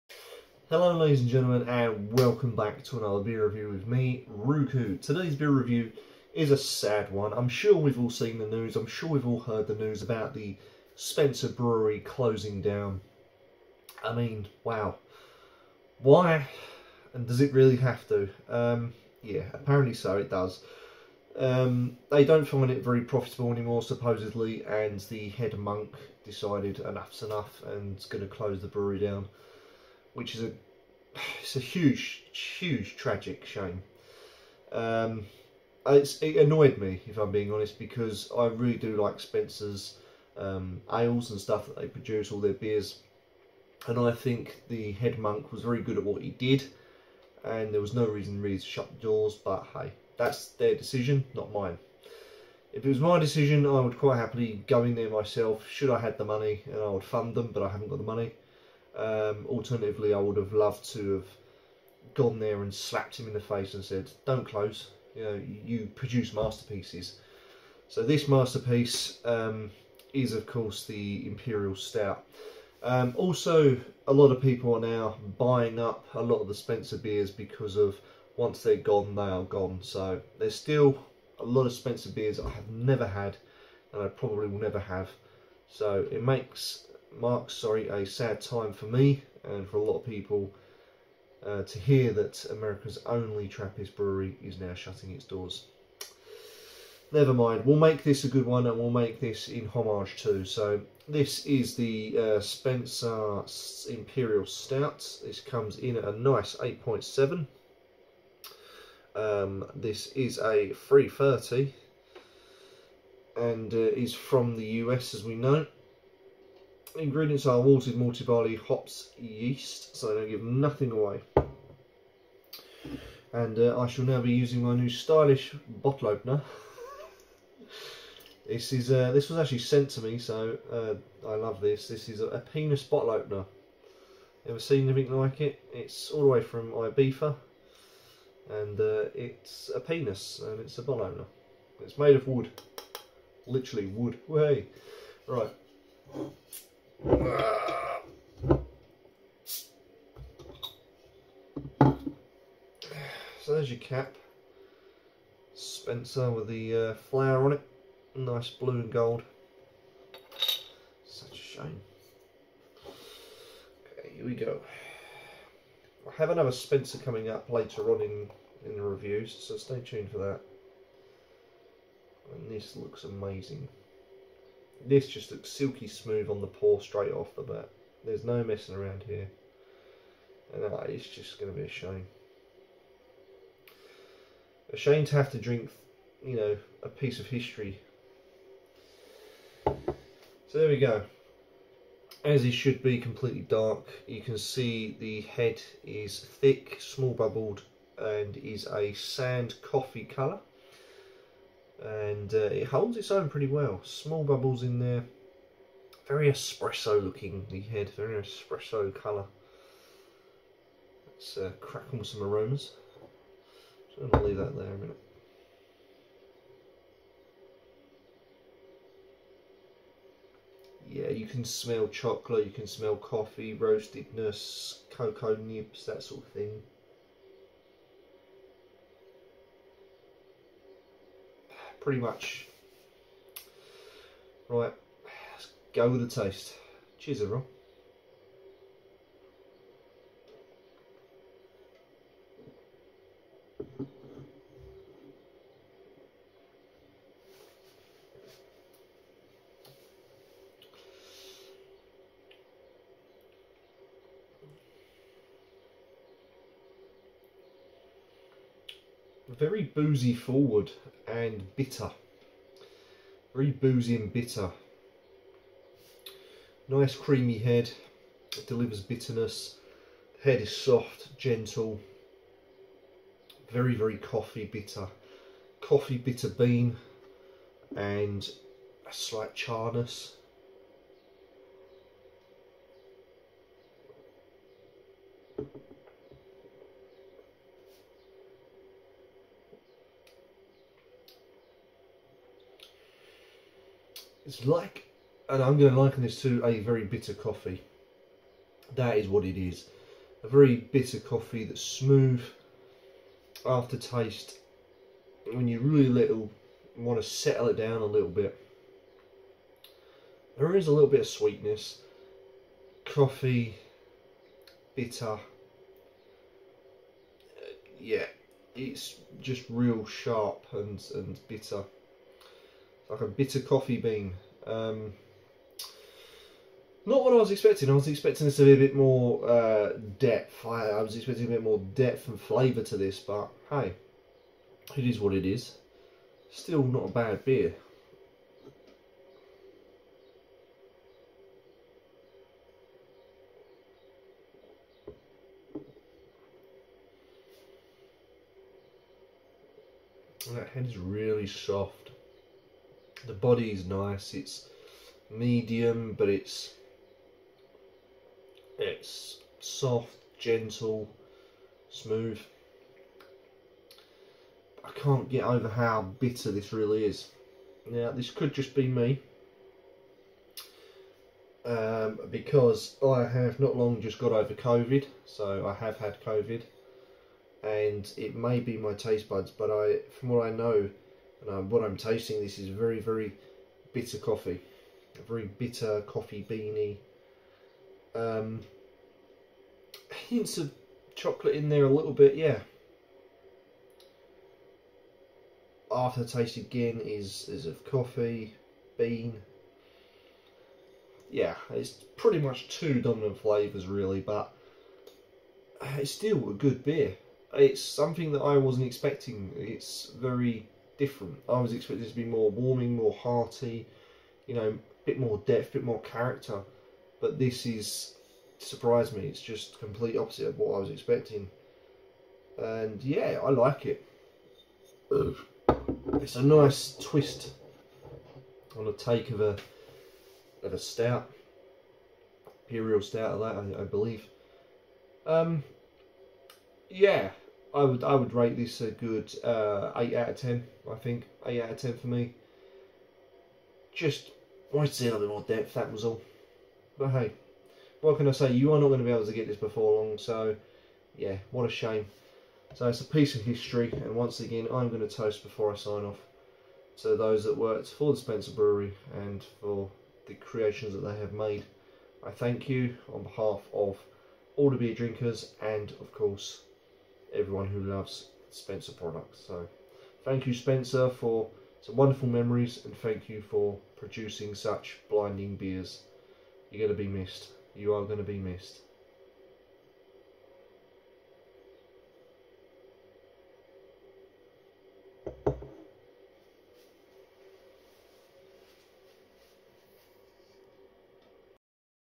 Spencer imperial stout review
sad news about the closing of the Spencer brewery, this is the Spencer imperial stout, bitter coffee through out this stout will always be remembered